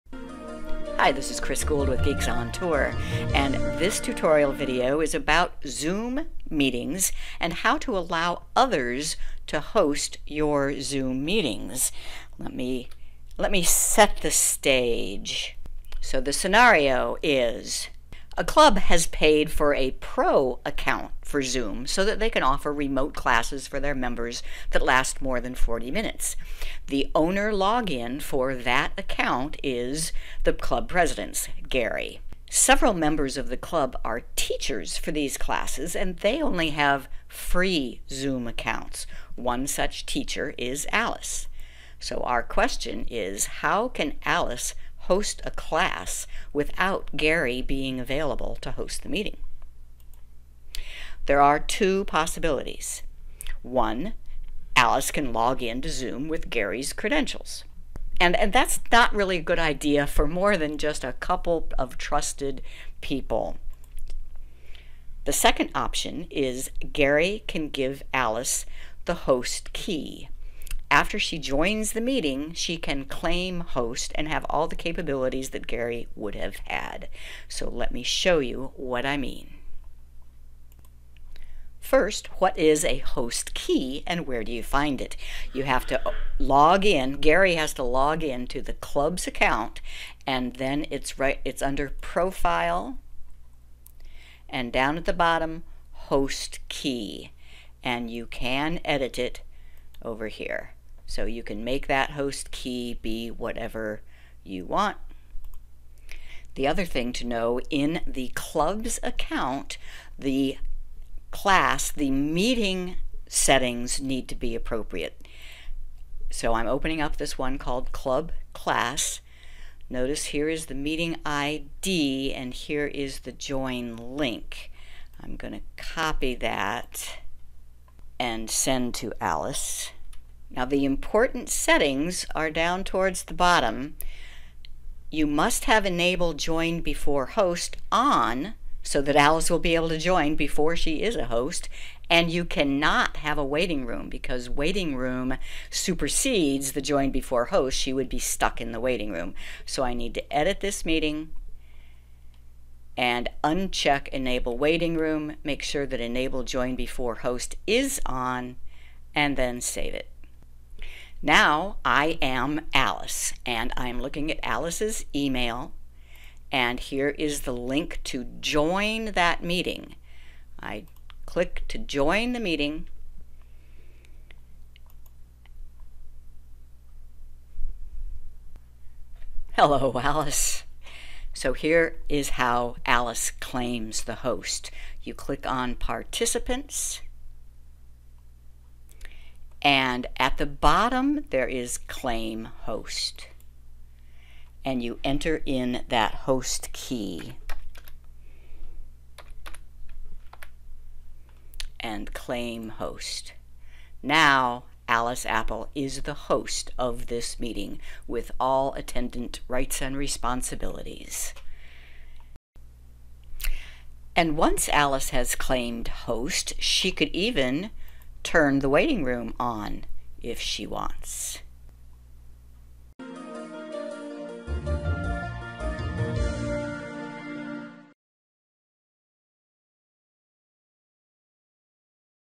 Zoom Meetings - How Do I Use Claim Host? Tutorial Video 629
A Host ID is a way to allow a scheduled meeting to be hosted by another attendee so they can control the meeting if the creator of the meeting can't attend.

You will learn how to allow an alternate host for a Zoom meeting if the original host cannot start the meeting.